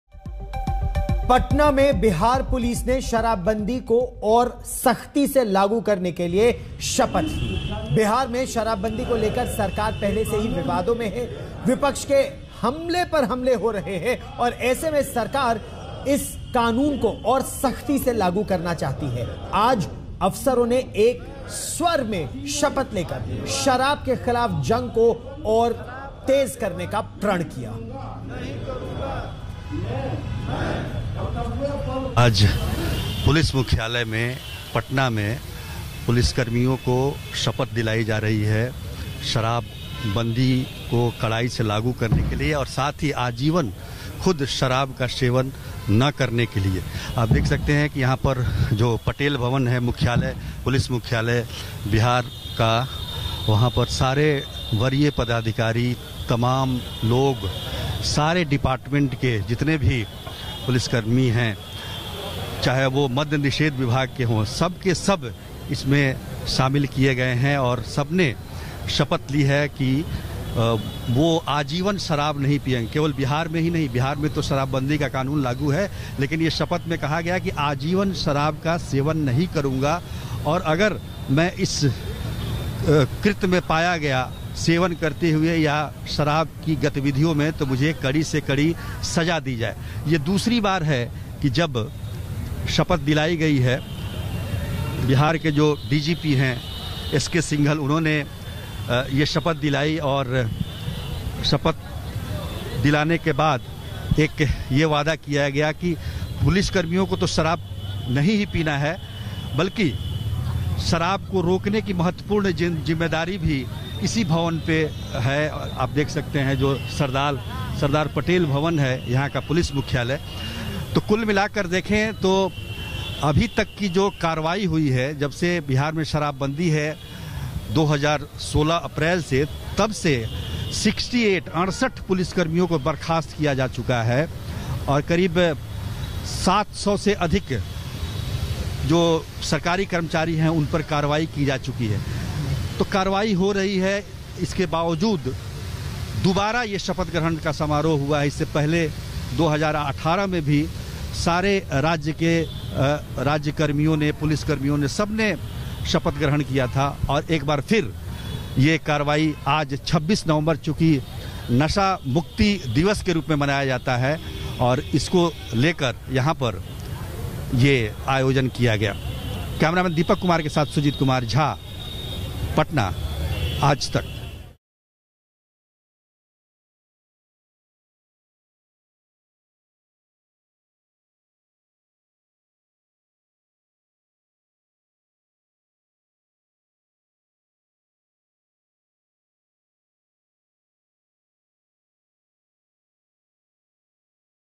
Action में Bihar Police, शराबबंदी के खिलाफ नया प्लान, 'शपथ' भी ली। Latest News। Hindi News
Bihar में शराबबंदी के खिलाफ Bihar Police ने कमर कर ली है. Bihar Police के अधिकारियों शराब सेवन ना करने साथ ही शराबबंदी को पूर्ण रुप से लागू करने की शपथ है. बता दें कि बीते दिनों Bihar में नकली-जहरीली शराब सेवन के बाद कई लोगों की जान चली गई थी.